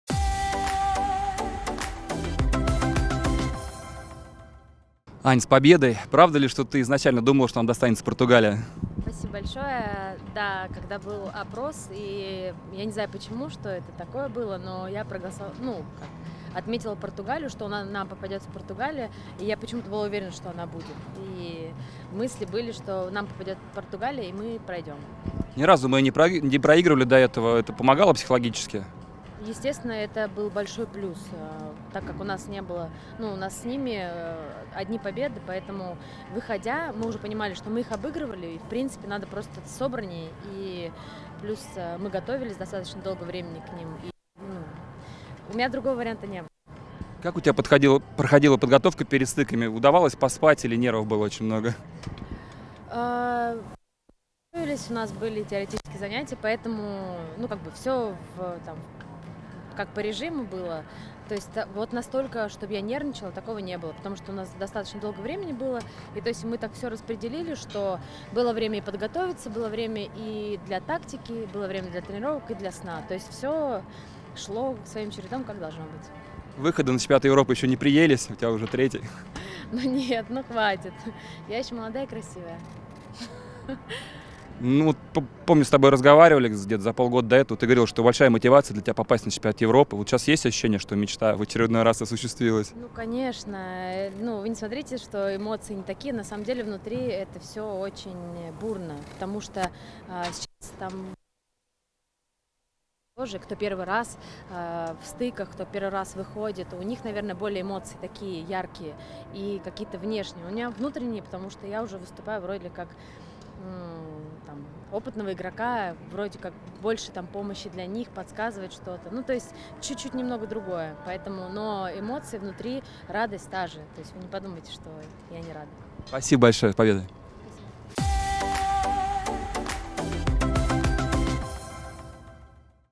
Анна Кожникова: "Знали, как пройти Португалию"
Капитан сборной России подвела итоги матча против Португалии, который вывел команду на ЕВРО.